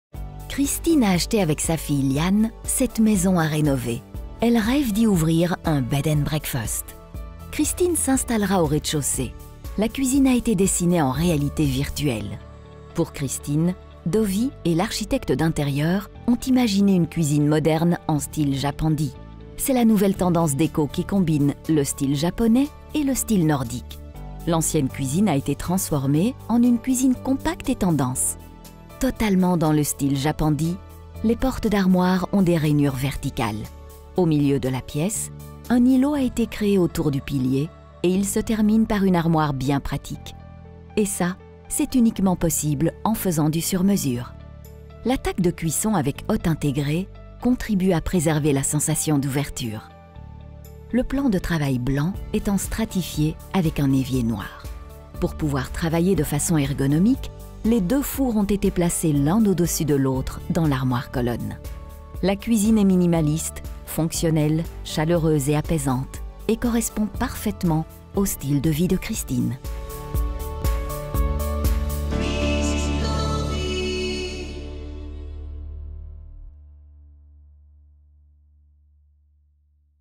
Christine profite de sa nouvelle cuisine en style Japandi | Cuisines Dovy
Christine et sa fille Lianne rêvent d'un bed & breakfast. Elles souhaitent vivre ensemble mais garder malgré tout chacune leur propre place dans la nouvelle maison. La maman Christine emménage au rez-de-chaussée. Elle souhaite un intérieur épuré, minimaliste et moderne avec une touche chaleureuse. L’expert-conseil cuisiniste de Dovy lui a recommandé le style populaire Japandi, qui est un style d'intérieur minimaliste avec des influences de la culture japonaise et du style de vie scandinave.